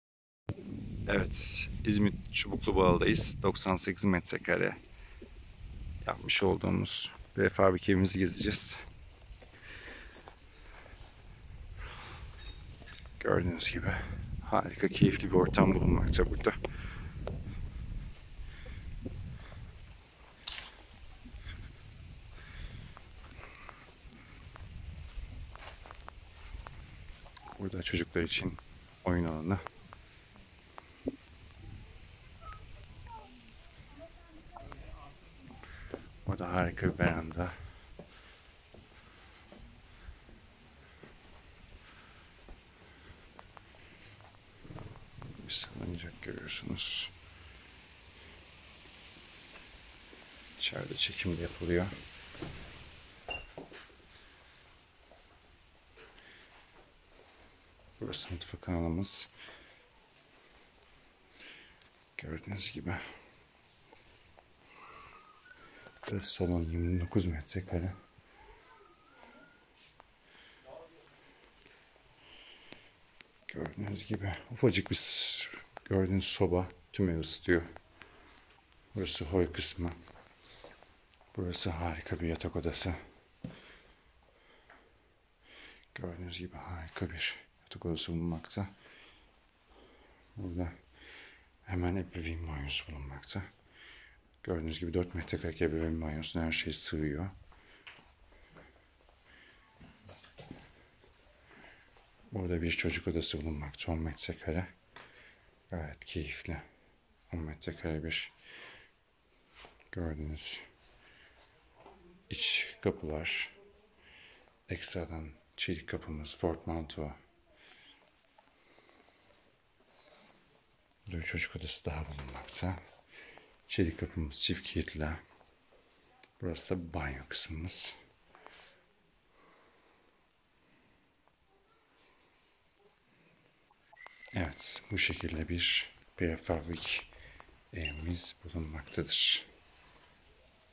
İzmit Çubuklubala Prefabrik Ev Gezisi
İzmit Çubuklubala da Ahmet Akbaş beyfendiye yaptığımız,
98 M2 - 3+1  Prefabrik Ev 🏘️
➡️ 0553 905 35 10 dan bize ulaşabilirsiniz.